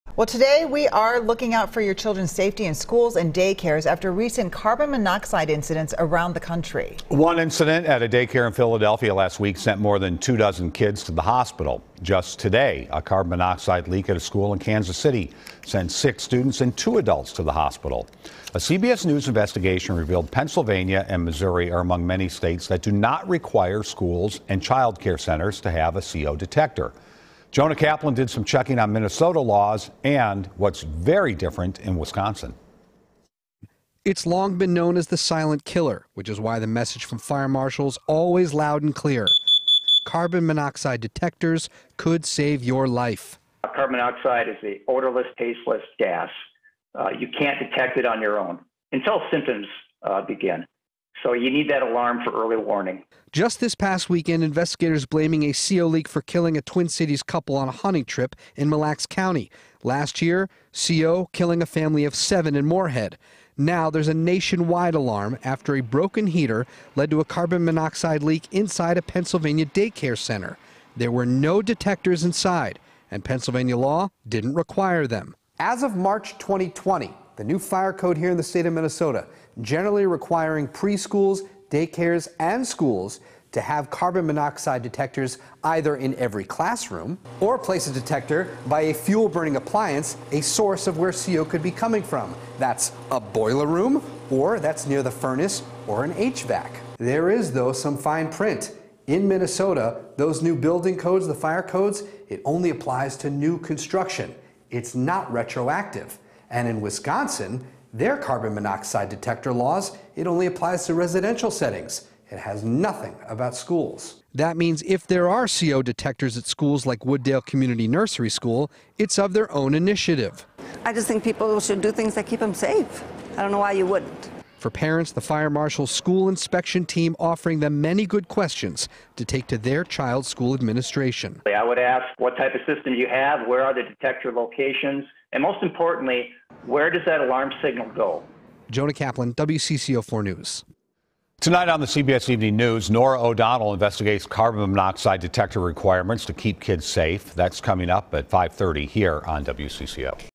Minnesota, Wisconsin laws differ on in-school CO detectors
A CBS News investigation revealed a number of states do not require schools and childcare centers to have carbon monoxide detectors.